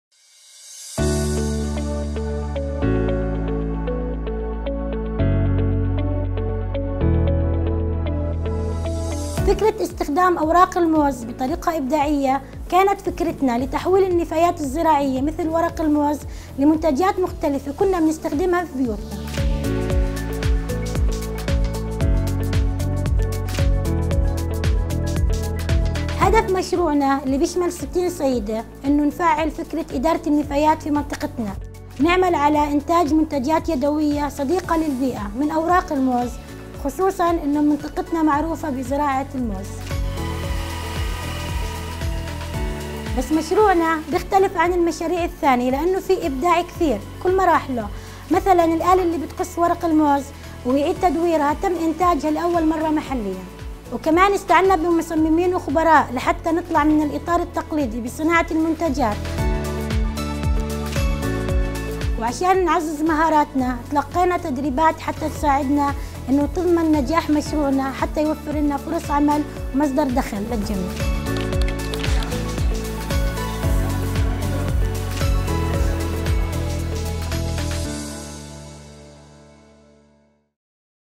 UNDP project Banana leavesmp4
Enhancing Women Participation in North Shouneh project

Targets 660 Women from Northern Shouneh
12 Green Businesses with Community Based Organizations
2 Sorting Stations and 1 Composting Plant Facility
120 Staff from the Ministry of Local Administration, Joint Services Council